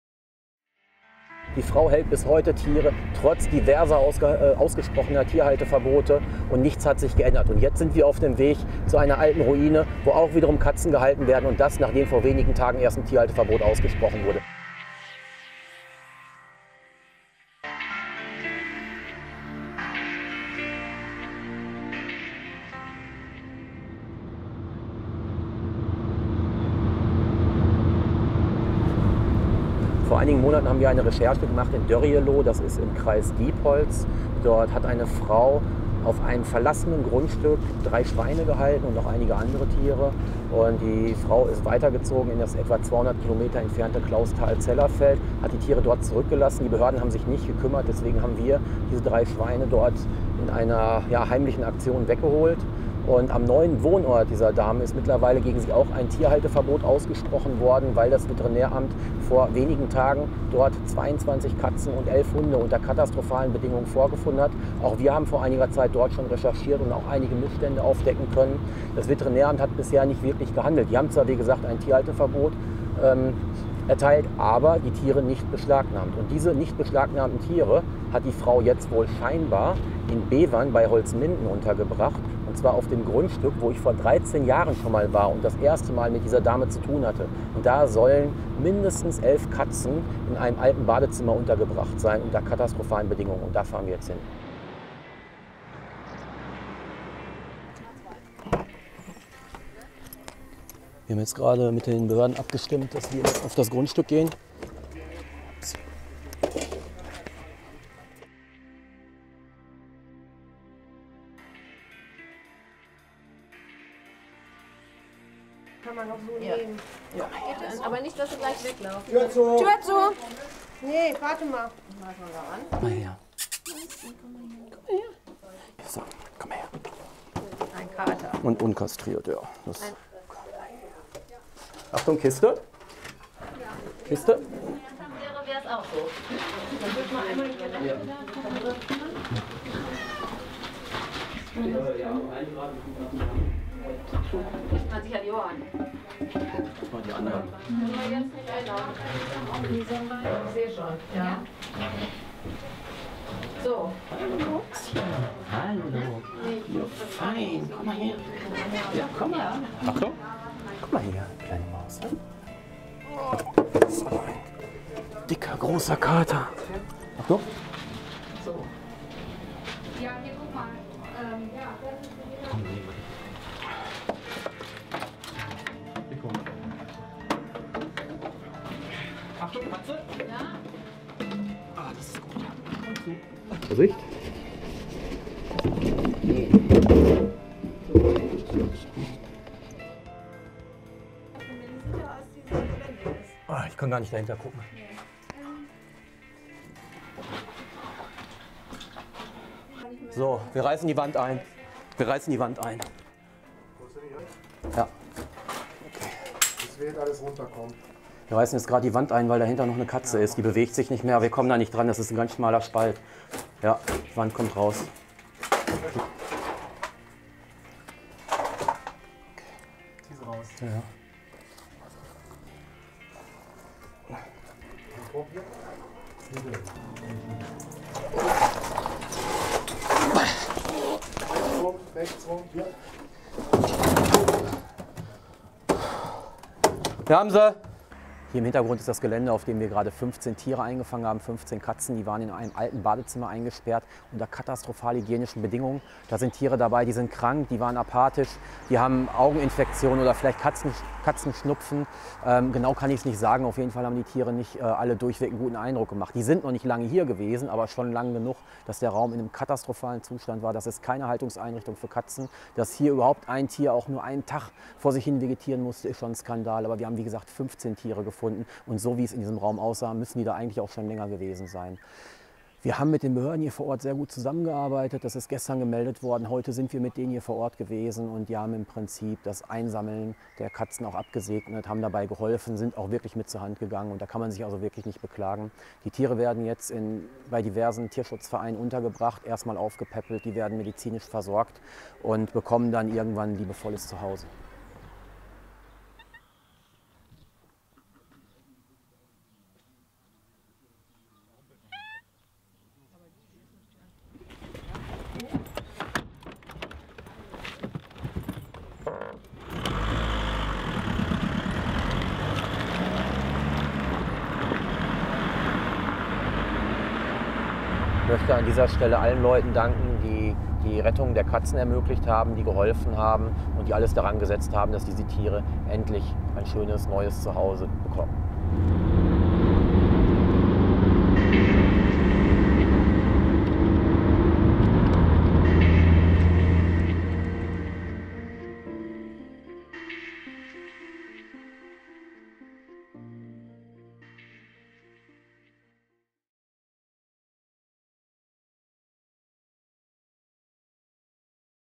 KATZENRETTUNG IN BEVERN // tierretter.de
Eine Durchgangsstraße in dem kleinen Ort Bevern bei Holzminden. Das Haus mit der Nummer 58 ist verlassen, heruntergekommen und eigentlich unbewohnbar. Dichtes Gebüsch lässt den Hinterhof eher wie einen Urwald wirken. Gewohnt hat hier schon lange niemand mehr. Doch im im Keller, in einem kleinen Badezimmer mit vielleicht sechs oder sieben Quadratmetern Grundfläche, fristen 15 Katzen seit einigen Tagen ihr Dasein. So gut wie alle Tiere haben Durchfall, es stinkt entsetzlich und es gibt weder eine richtige Futter- oder Wasserversorgung, noch ein vernünftiges Katzenklo für so viele Tiere. Die Haltung der Katzen ist unwürdig, rechtswidrig und in jeder Hinsicht unbeschreiblich grausam.

Einige Tage zuvor haben Nachbarn die bekannte Animal Hoarderin Barbara B. beobachtet, wie sie etwas ins Haus gebracht hat. Kurz darauf stellen Tierschützer fest, dass schätzungsweise 11 Katzen im Keller vor sich hinvegetieren. Die Behörden und der Verein tierretter.de e.V. werden umgehend eingeschaltet.